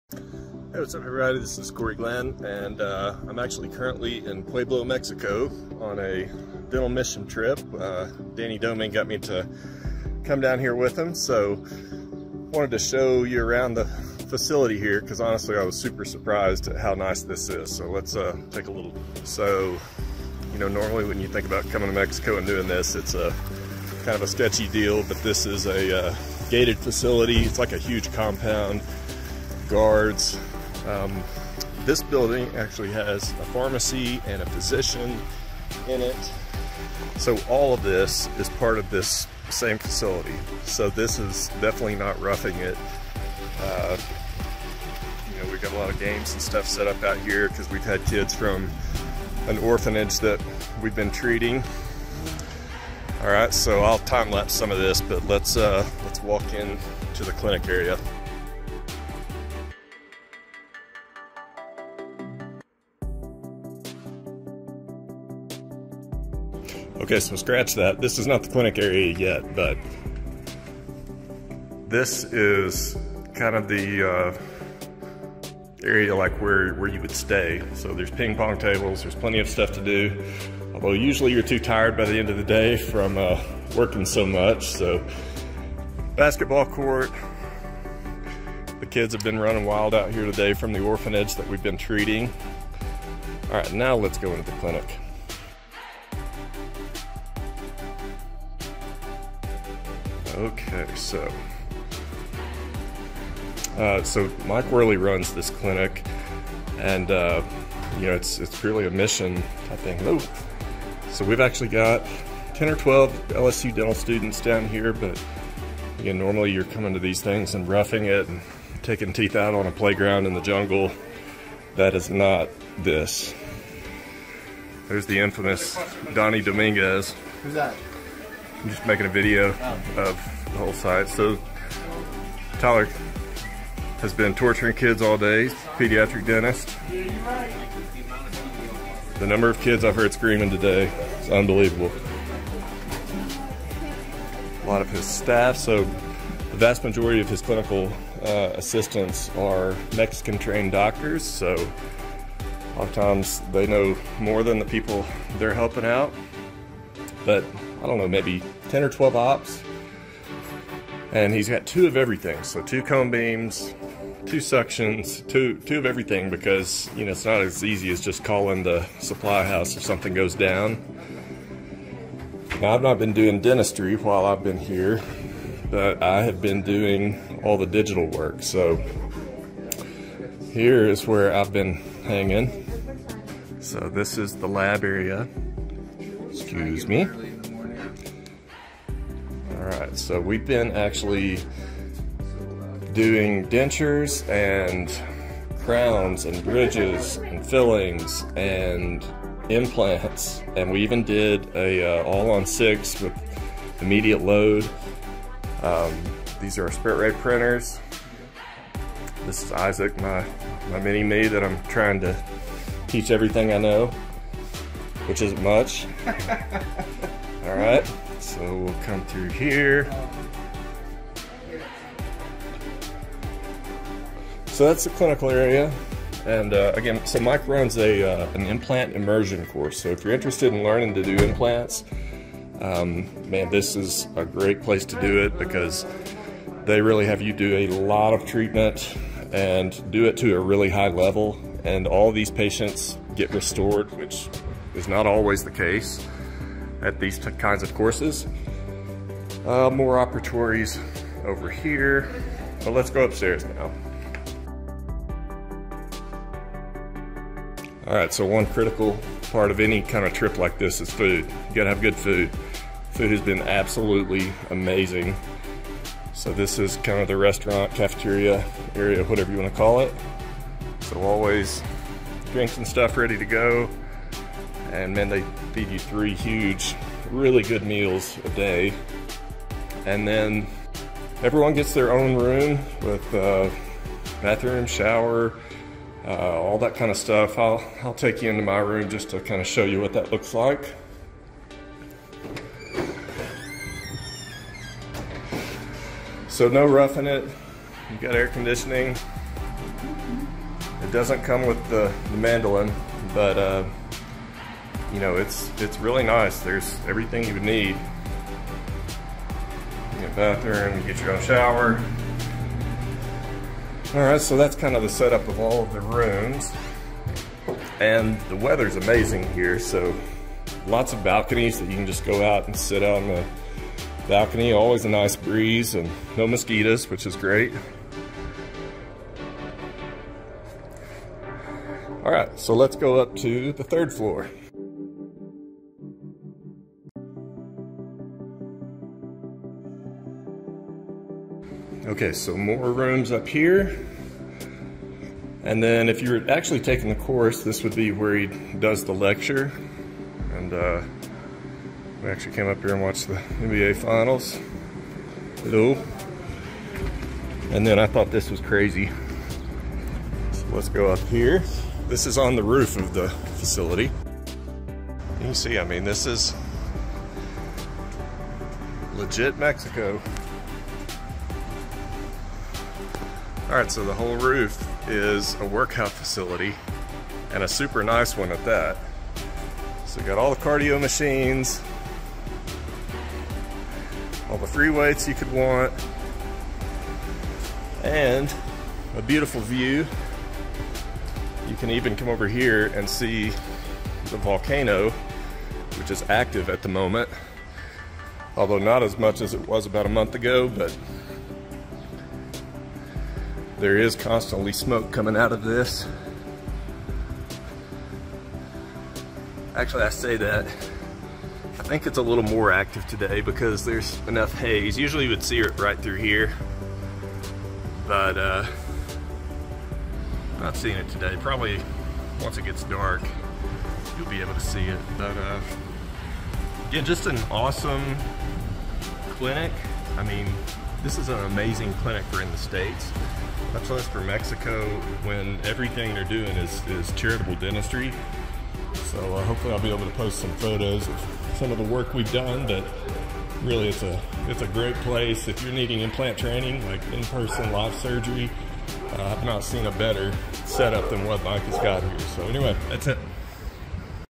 Wehrle Implant Immersion Course Facility Walkthrough
This was recorded while I was on a dental mission trip to Pueblo Mexico.  Mike Wehrle was gracious enough to let us use his clinic and to let us stay on site.  While we were not there for this reason, Mike runs live patient implant training courses if you are interested in learning to place implants.